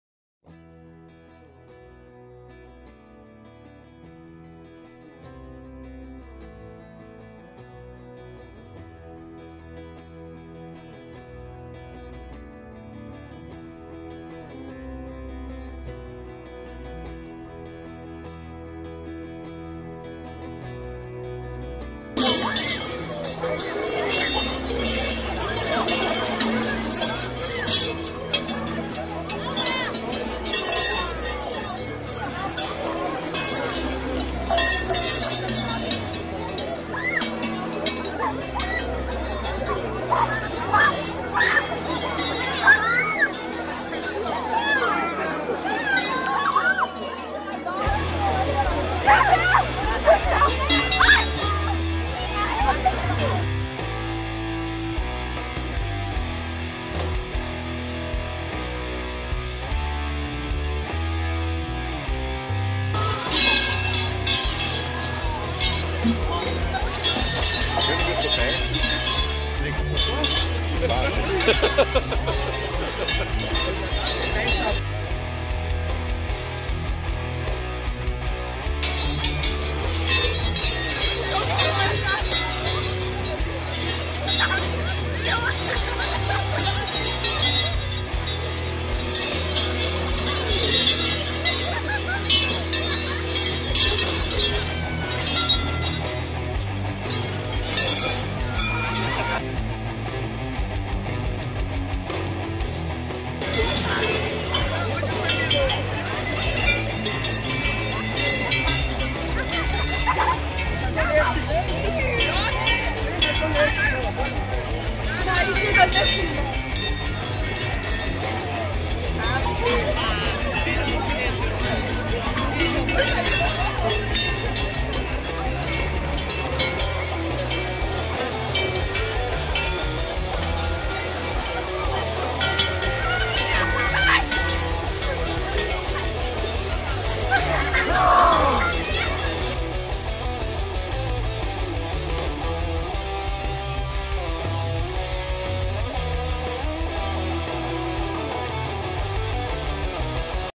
Adventnachmittag Ludmannsdorf 2018
Krampusgruppe Wellersdorf am Adventnachmittag in Ludmannsdorf 2018